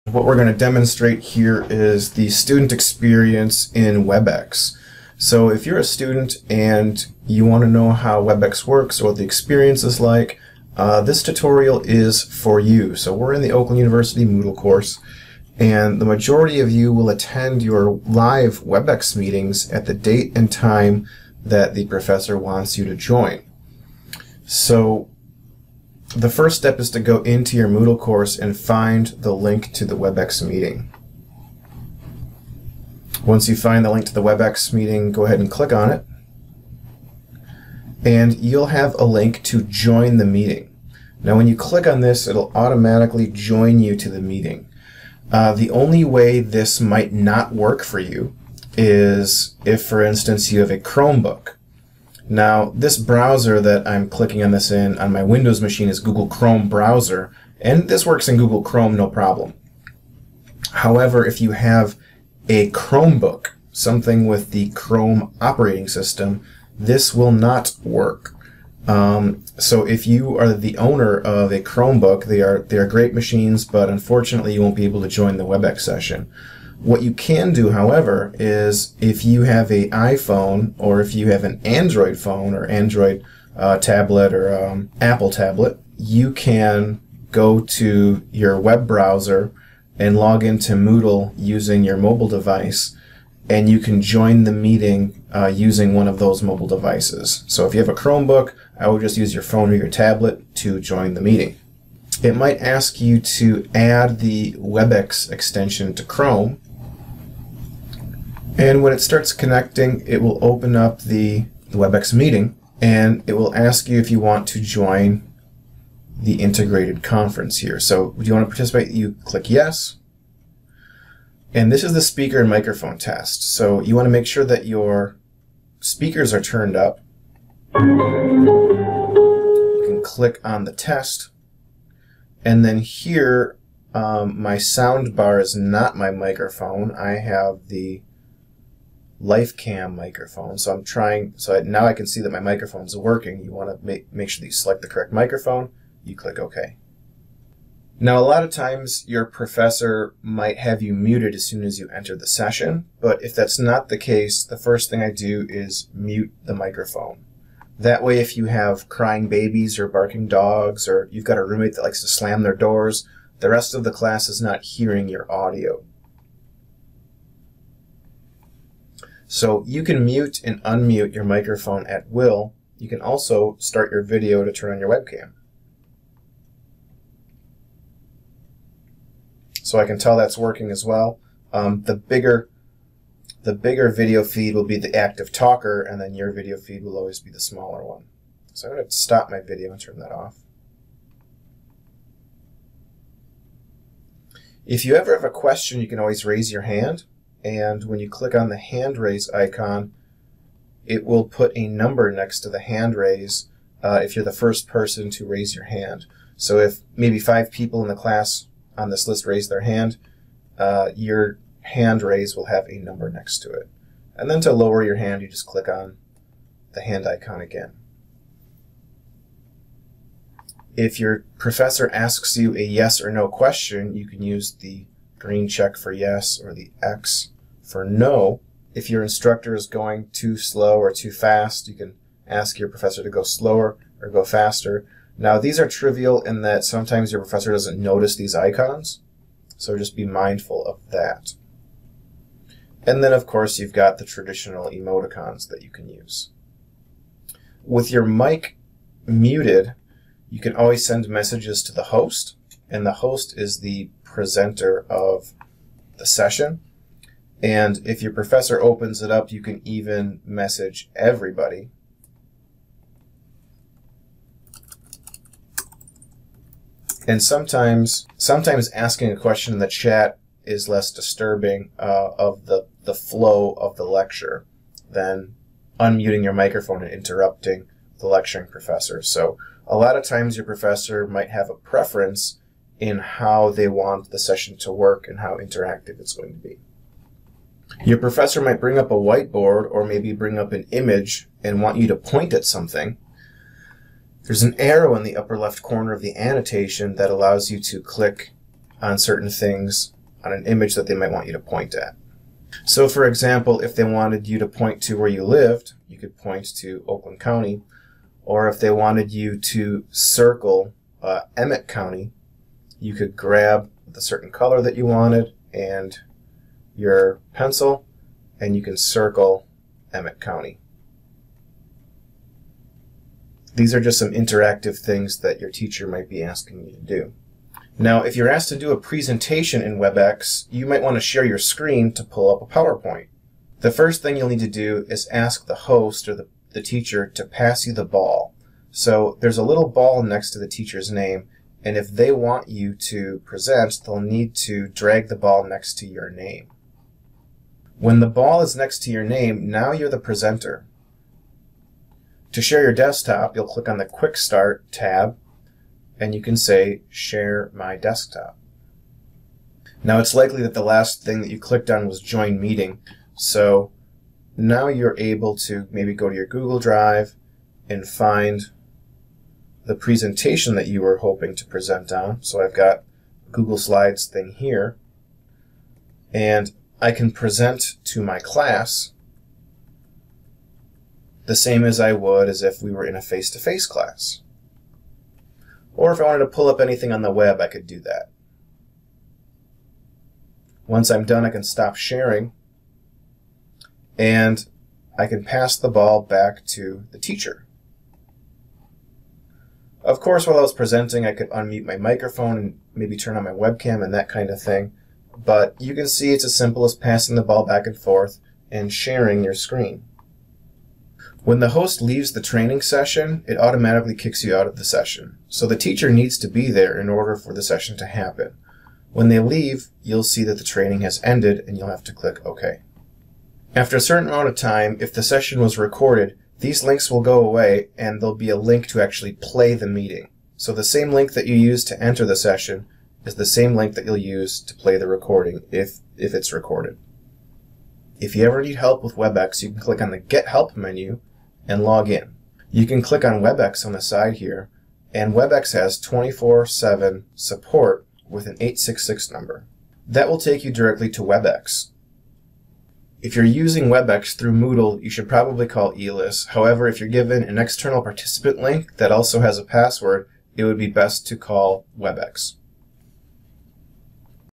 Students using WebEx - Student Tutorial - Oakland University Moodle
This demonstration shows how student Join and navigate WebEx meetings.